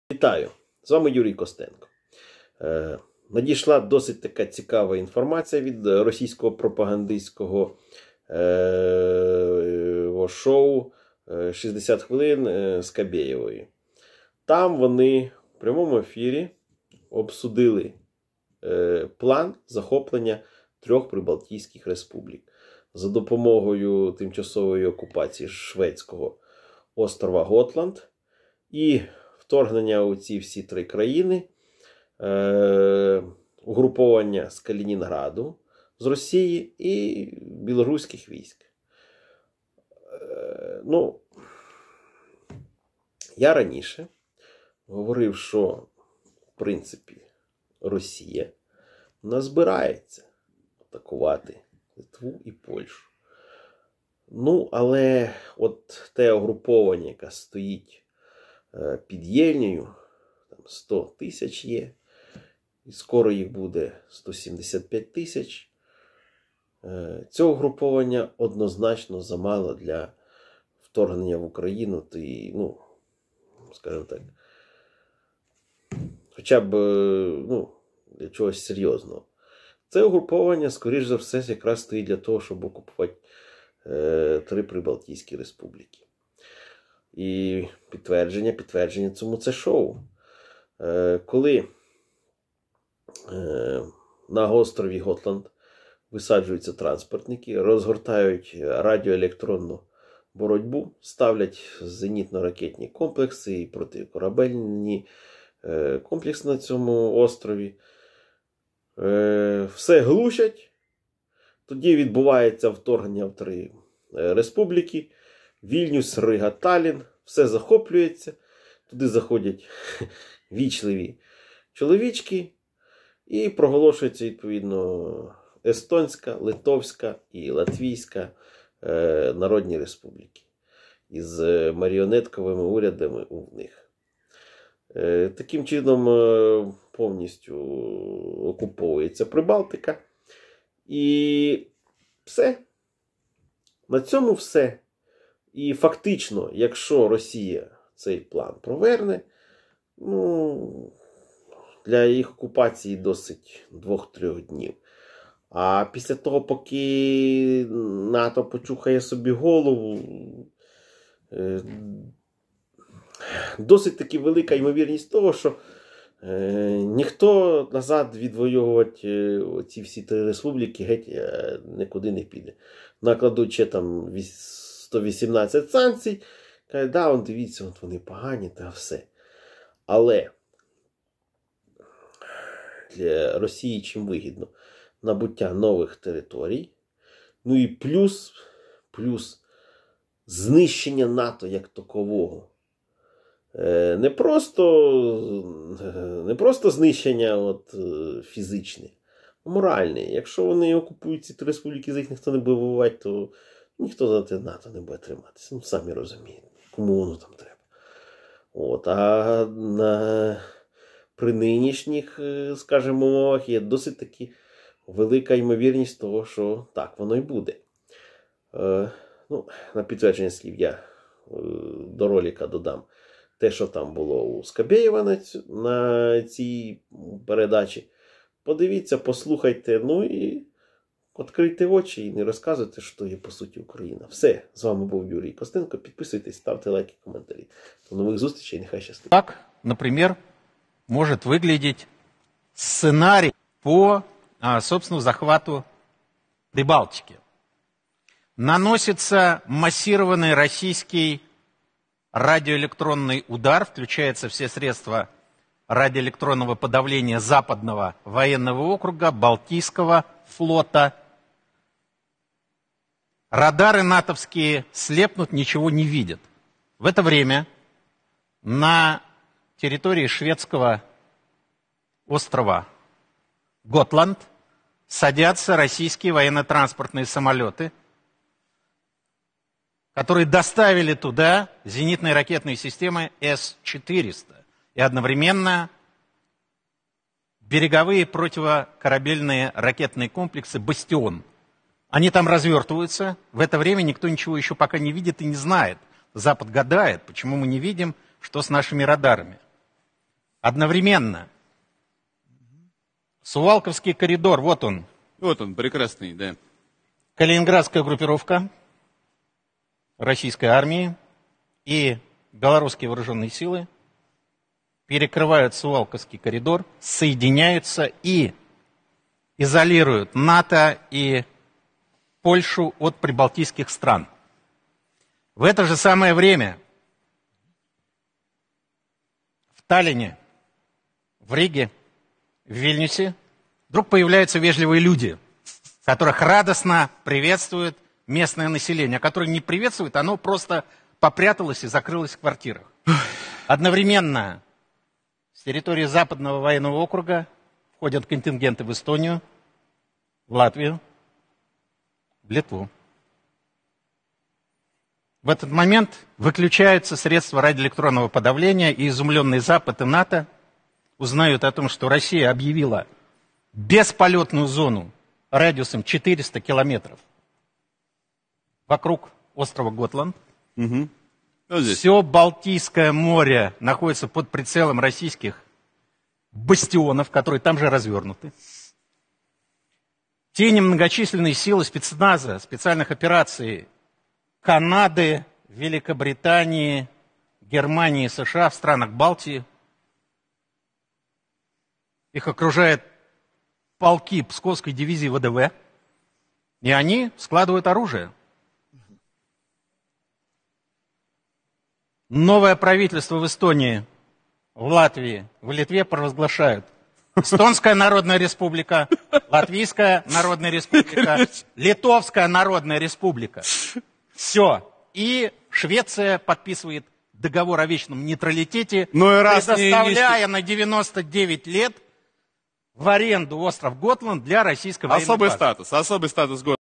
Захват Прибалтики Россией: план и подробности
План захоплення Прибалтики Росією.
Якщо вам подобається те, що я роблю і навіть якщо не подобається— буду вдячний за фінансову підтримку мого проекту. 💙💛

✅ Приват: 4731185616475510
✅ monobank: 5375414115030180